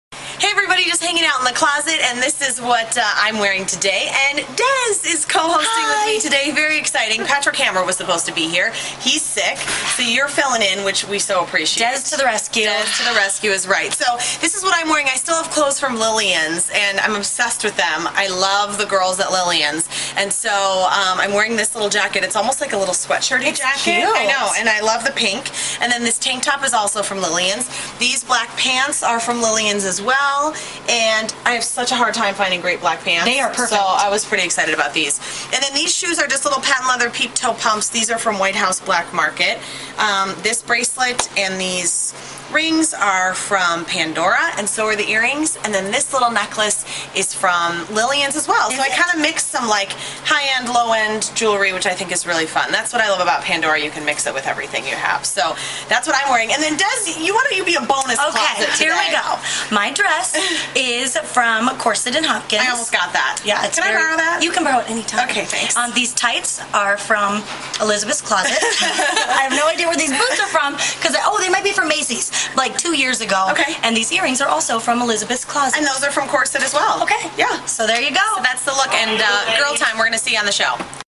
Elizabeth Ries Closet Dec. 14, 2010 With Desiree Dez Gravelle Version 2
Elizabeth Ries:
Jacket, Tank Top, Pants/Necklace: Lillians
Other Jewelry: Pandora
Shoes: White House Black Market
Desiree Dez Gravelle:
Dress/Earrings: Corset in Hopkins
Tights: Elizabeth's Closet :)
Boots: Macy's